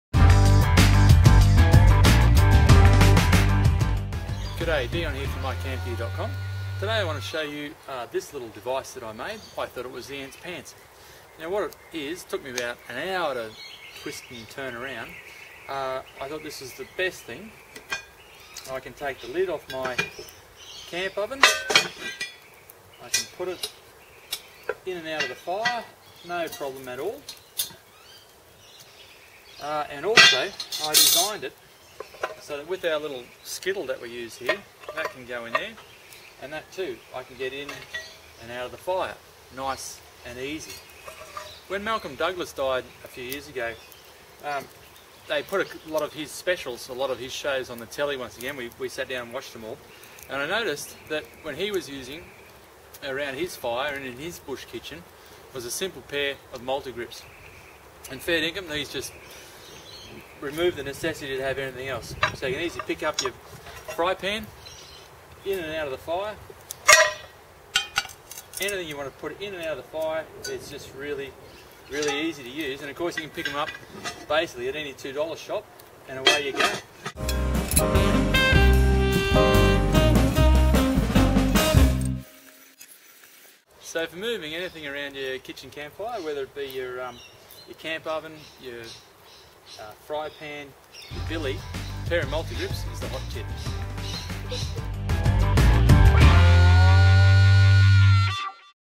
Multigrips in the camp kitchen - best tool ever
Cooking in the camp kitchen joust got a whole lot easier with a pair of multi grips